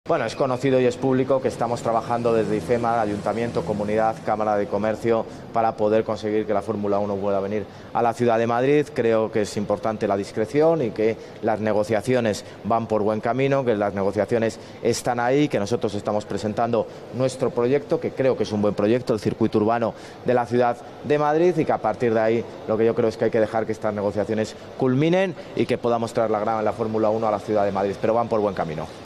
Almeida dice que las negociaciones para traer la F1 "van por buen camino"
El alcalde de Madrid, José Luis Martínez-Almeida, que ha pedido "discreción en las negociaciones", ha afirmado que las negociaciones para acoger un Gran Premio de F1 "van por buen camino".